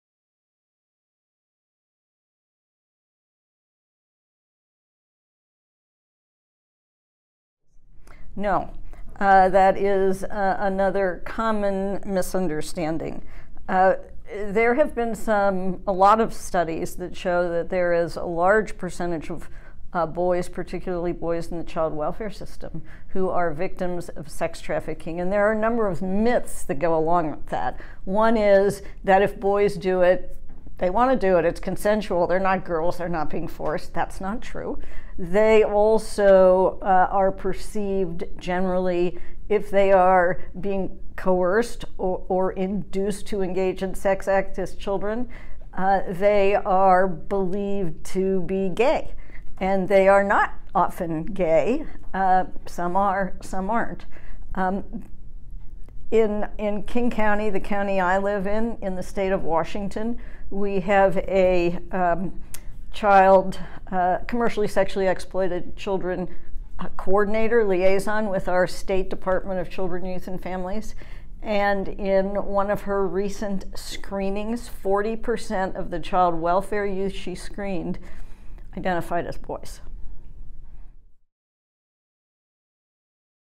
Does domestic child sex trafficking only affect girls?
January is National Slavery and Human Trafficking Prevention Month. NCJFCJ's Hon. Barbara Mack (Ret.) dispels the myth that only girls are affected by domestic child sex trafficking. For more information about the National Council of Juvenile and Family Court Judges (NCJFCJ),